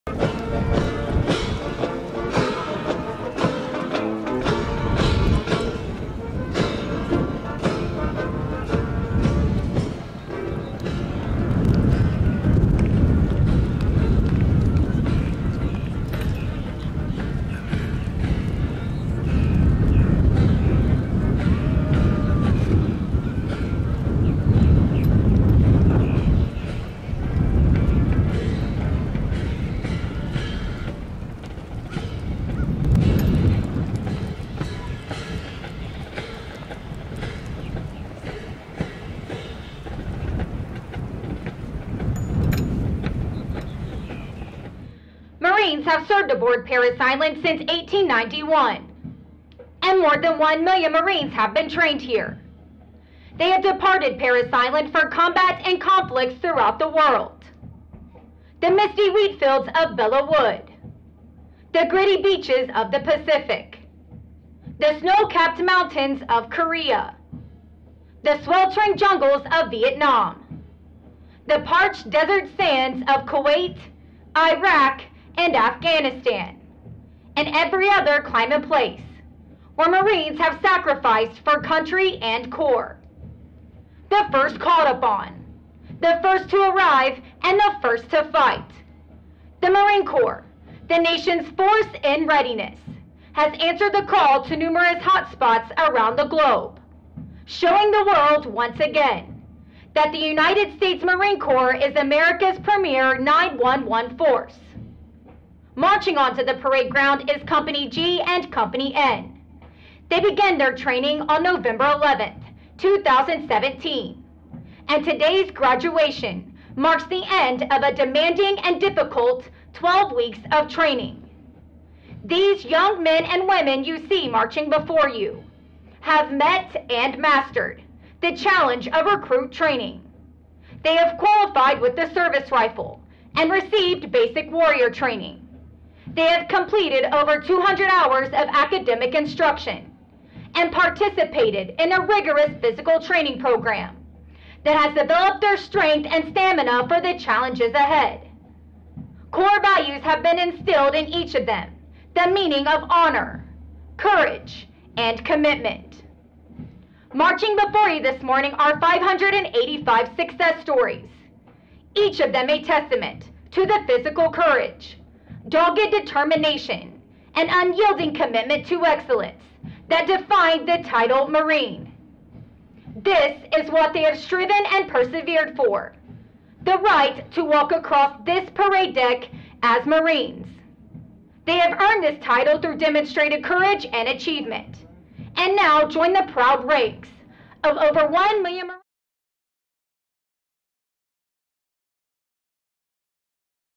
Golf & November Company Graduation - 02/02/2018
After 3 months of hard training the Marines of Golf and November company march on the parade deck for the graduation ceremony on Parris Island.